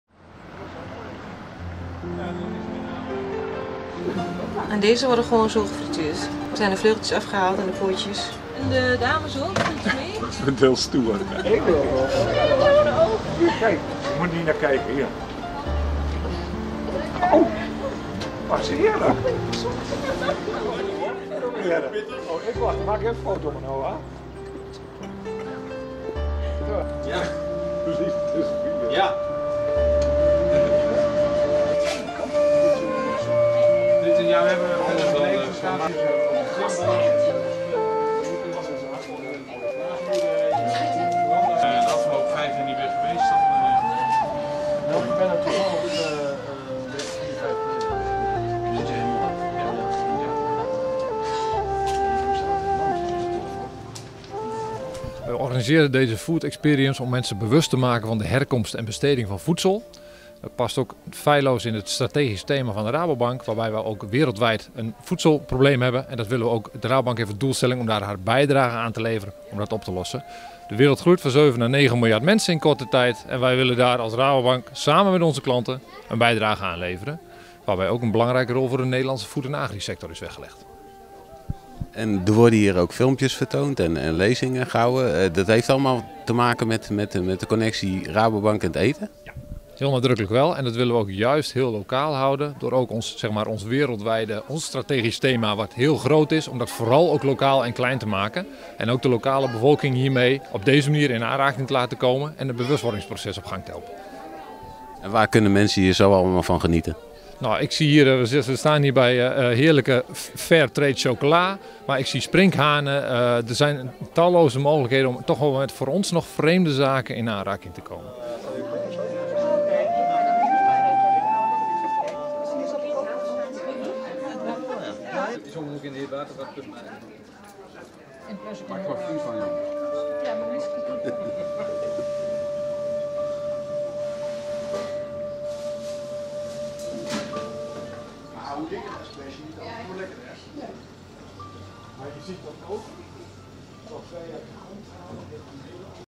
Rabo Food Experience
De Rabobank wil mensen bewust maken, waar het eten vandaan komt en dat de groei van de bevolking ook een bewuster omgaan met eten zal inhouden voor de toekomst.